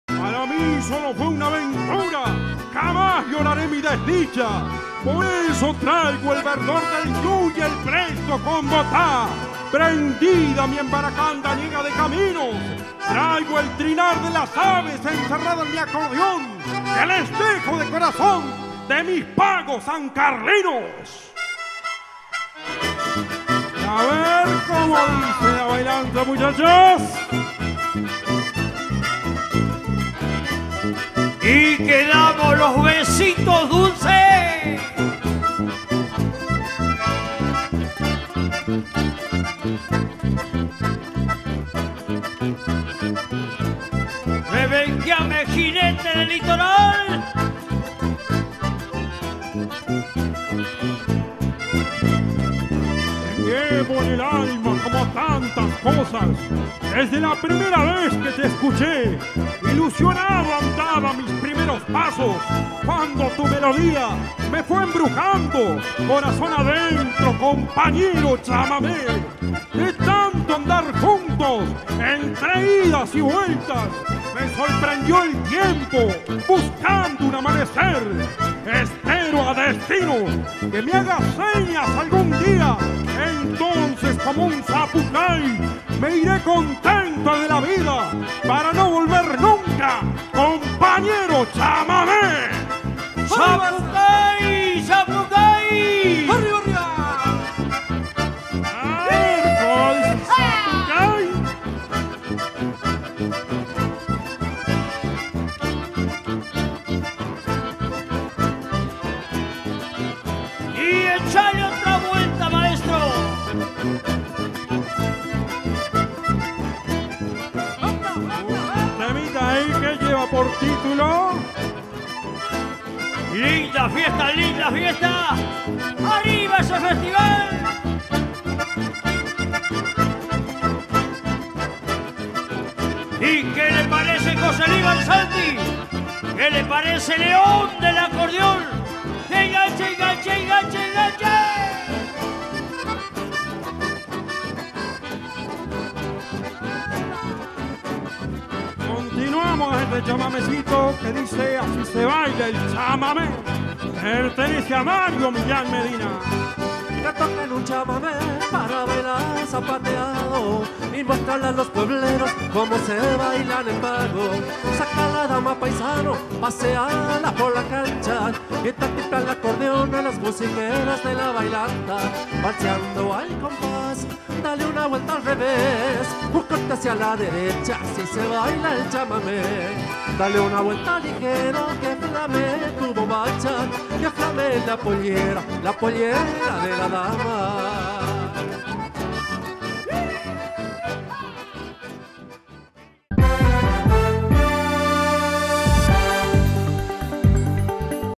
Actuación de los Jinetes del Litoral
Se presentaron en el San Carlos Canta con Dulzura